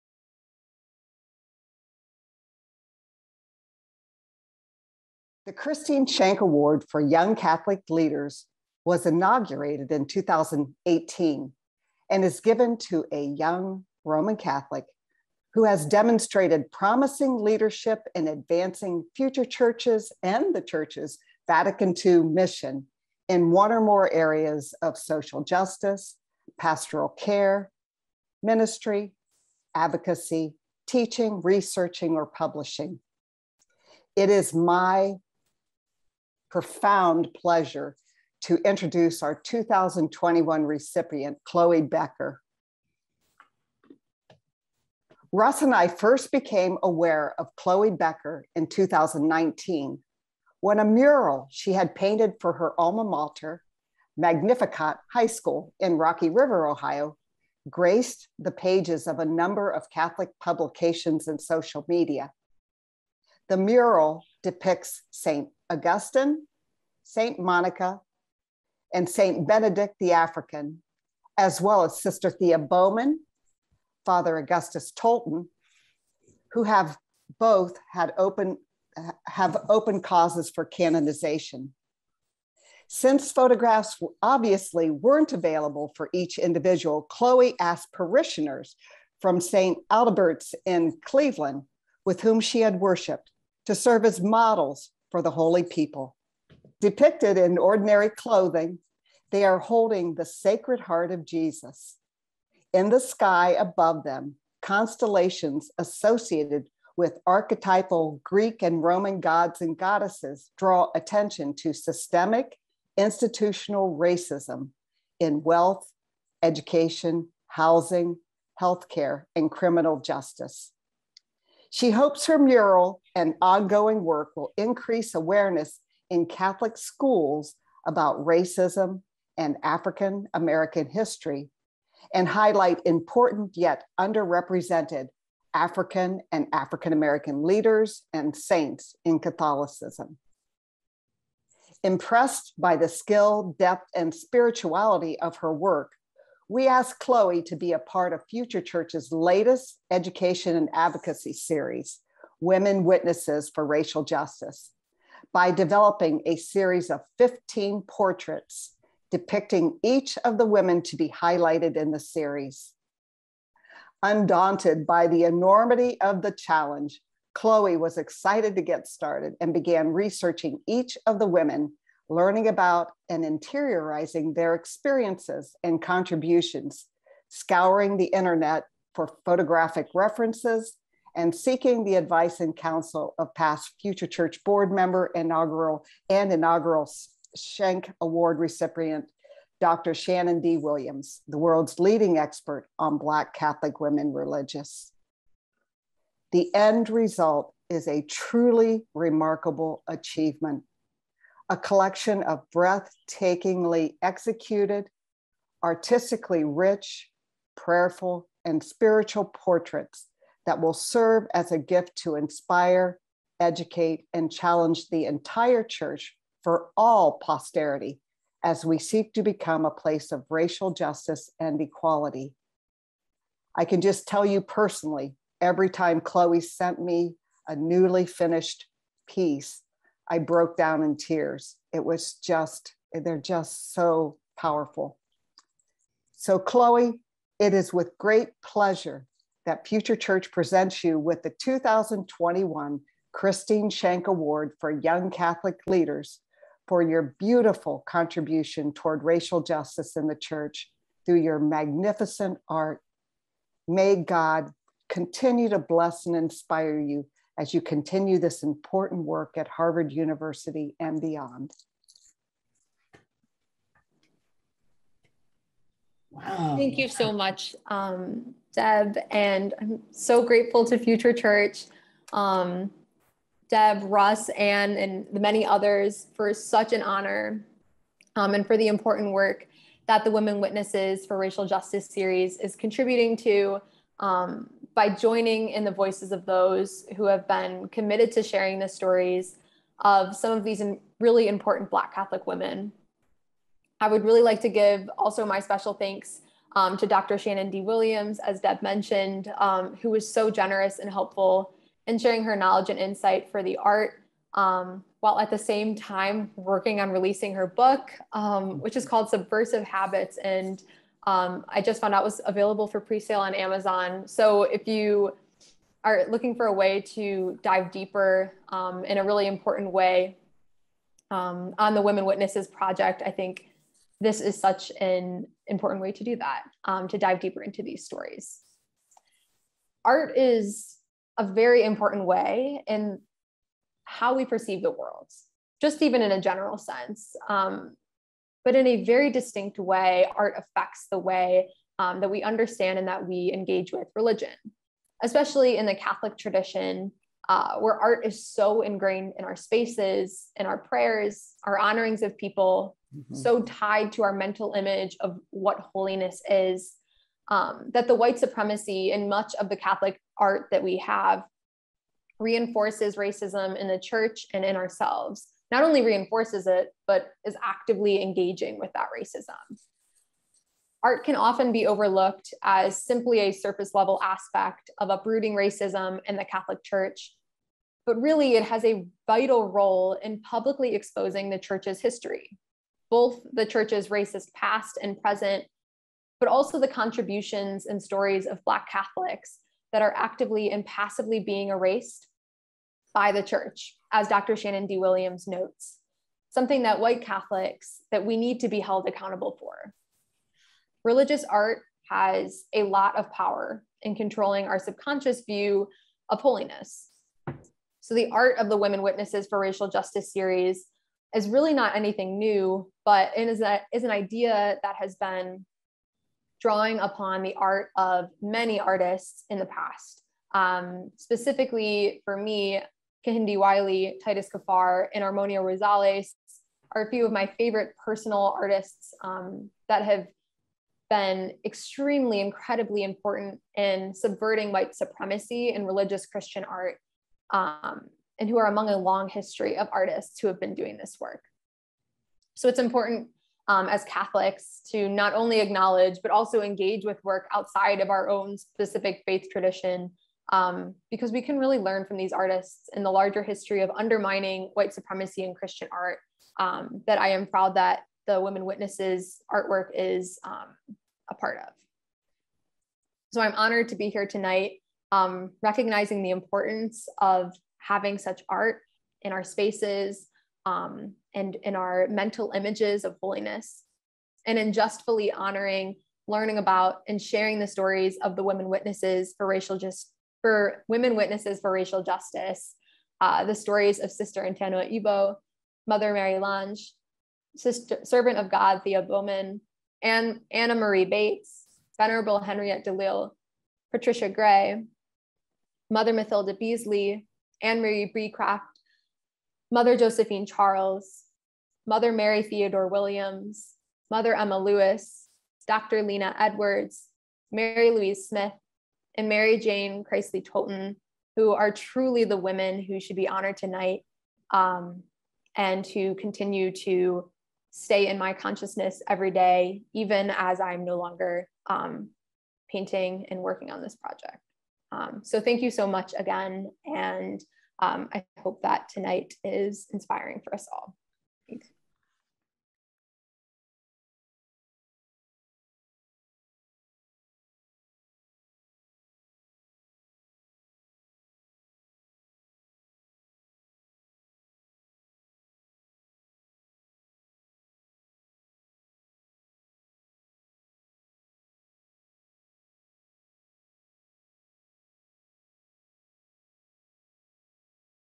Chloe Becker Accepts the 2021 Christine Schenk Award for Young Catholic Leaders
FutureChurch presents Chloe Becker with the 2021 Christine Schenk Award for Young Catholic Leaders for her contribution toward racial justice in the Church through her magnificent art.

for Chloe's Women Witnesses for Racial Justice Series.